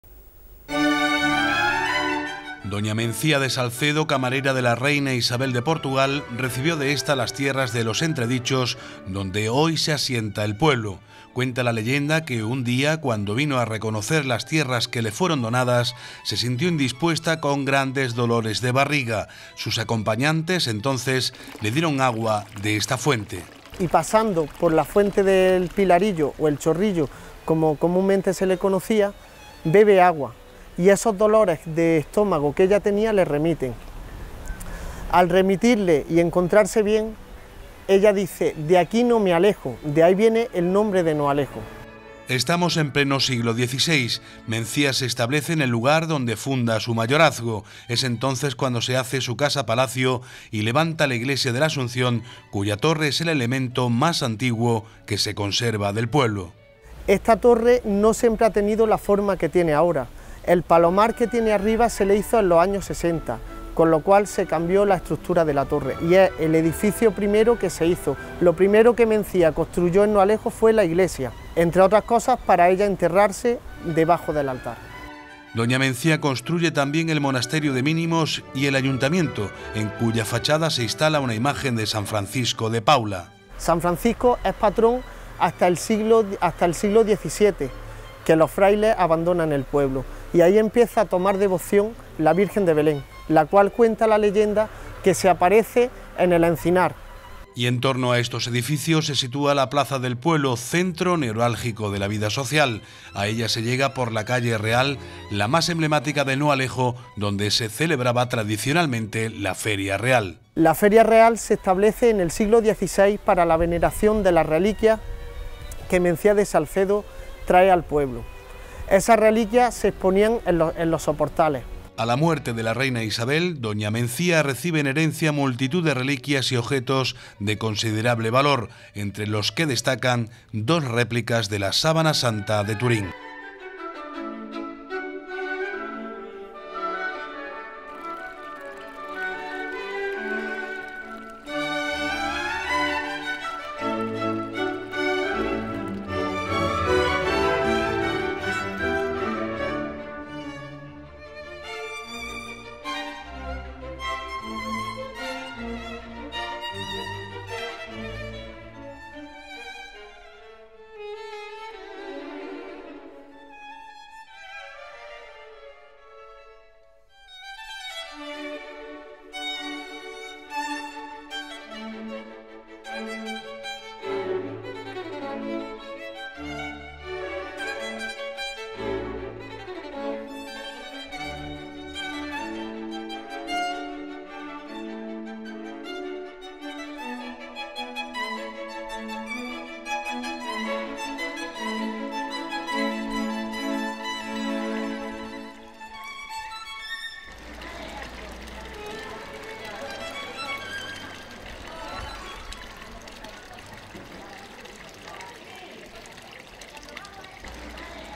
Antiguas fotografías nos trasladan a Noalejo y al origen de su nombre
A veces, la historia y la leyenda mantienen una complicidad que perdura en el tiempo. Ha sucedido así con el origen de Noalejo, un municipio del que rescatamos hoy viejas fotografías que nos trasladan a un lugar, la Fuente del Pilarillo. Allí, según la tradición, surge el nombre del pueblo...